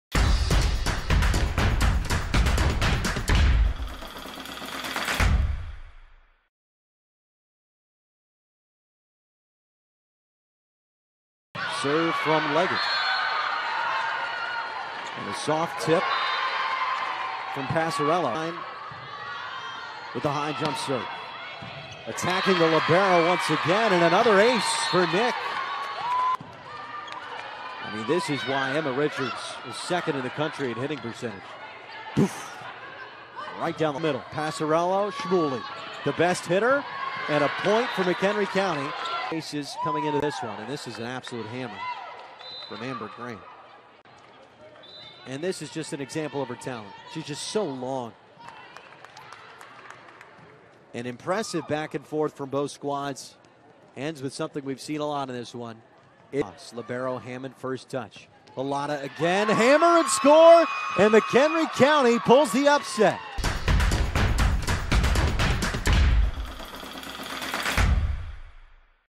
2019 NJCAA DII Volleyball Championship - Gulf Coast State vs. McHenry County
No. 11 McHenry County scored the first upset of the 2019 NJCAA DII Volleyball Championship with a 3-2 win over No. 6 Gulf Coast State in Charleston, WV